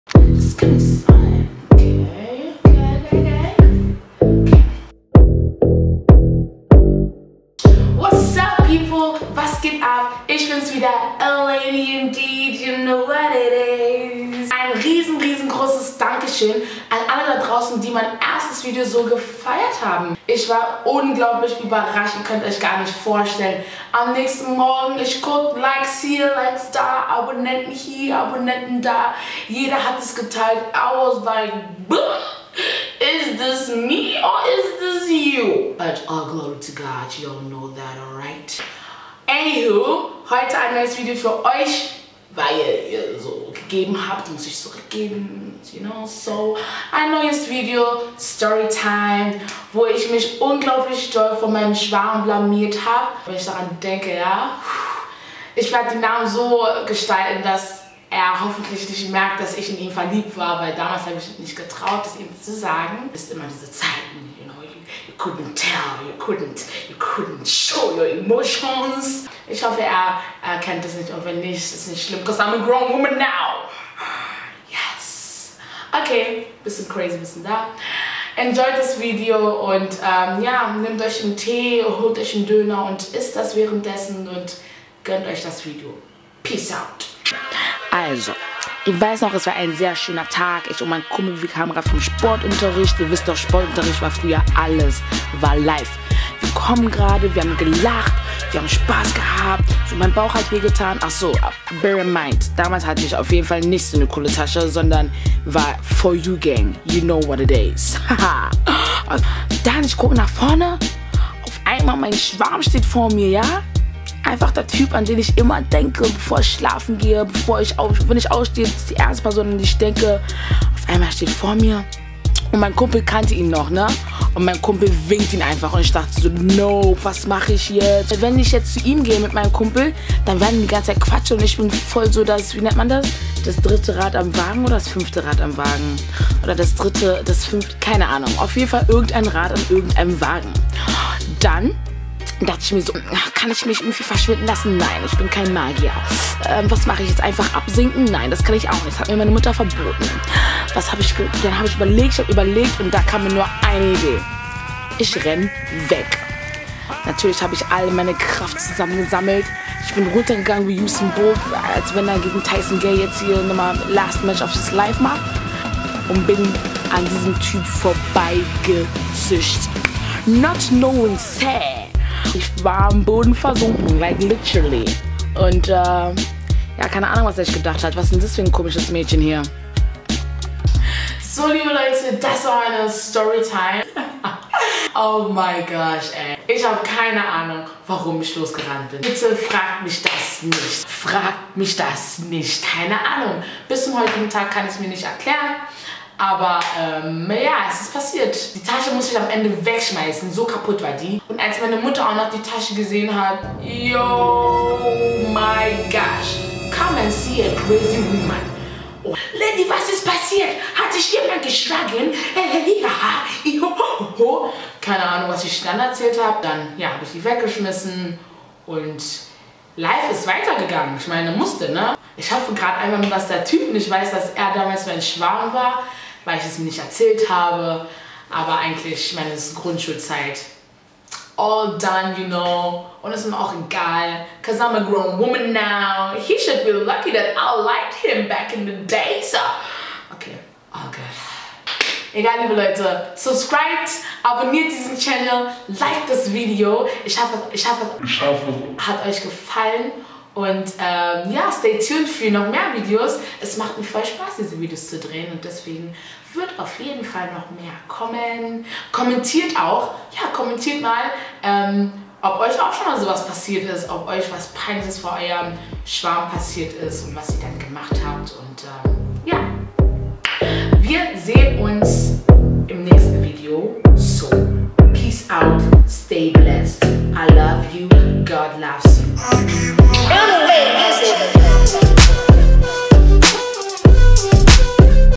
STORYTIME | MEGA PEINLICHE AKTION !!!
Hey Leute das ist mein zweites YouTube Video (Storytime) indem ich von einer peinlichen Story erzähle, ich hoffe es gefällt euch.